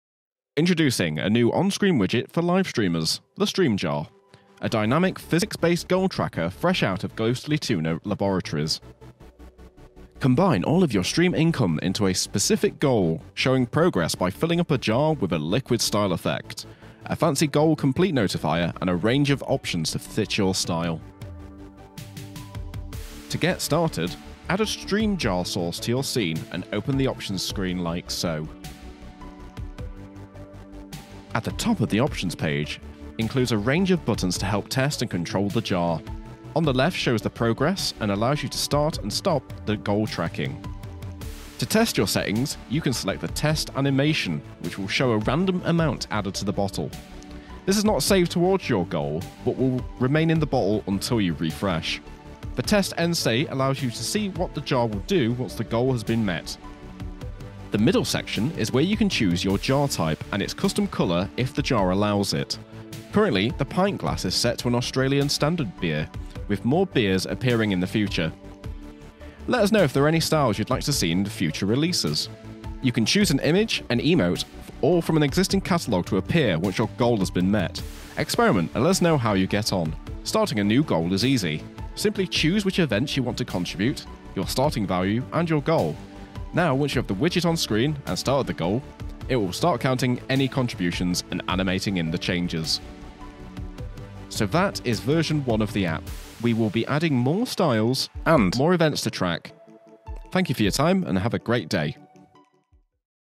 Stream Jar - Streamlabs Desktop Version
Combine all of your stream revenue into one goal!

Track your progress with this fancy overlay widget, choose which events contribute and see the goal jar fill up as you go!

Got any questions or feedback?

Check out some of my other work!